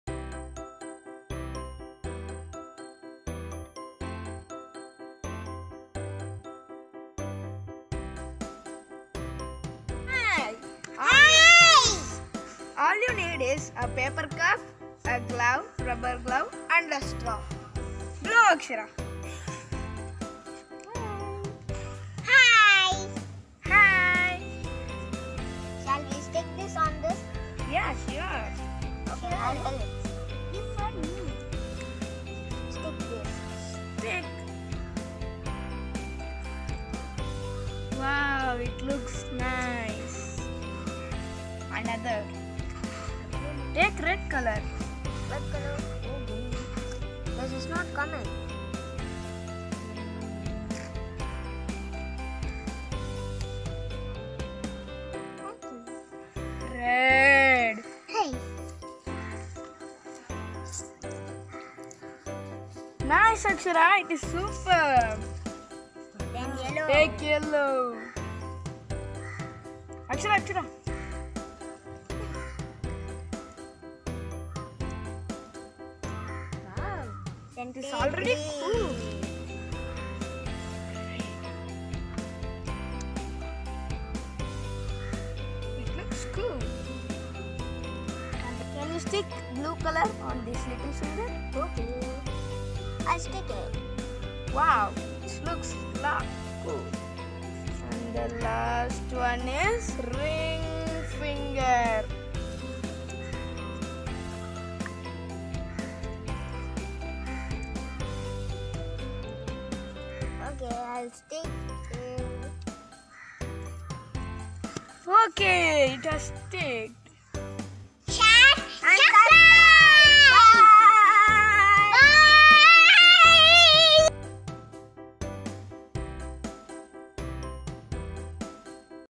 DIY Hand Puppet
Akshu enjoying a puppet made using Cup, Straw, Hand Glove and decorated by stickers. We made this outdoor so that kids love playing outdoors. Akshu, Sai and Adharsh having fun outdoors playing with this puppet.